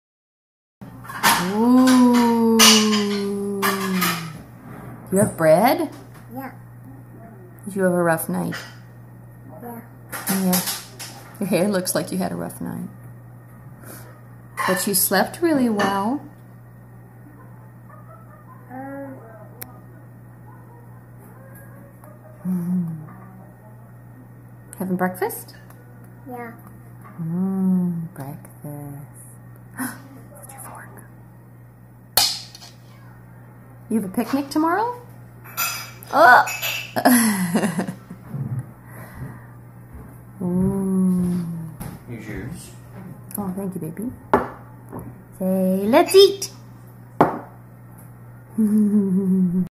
LELLIVENTURES! Rough night? Breakfast bread
She was so cuuute! Lelly looks like she went on a bender... XD but pastries more than make up.

Adventures with Lelly in Japan! Our village in Unzen City, my family's hometown as of 2013 while I am with the JET Programme - Nagasaki Prefecture, Japan.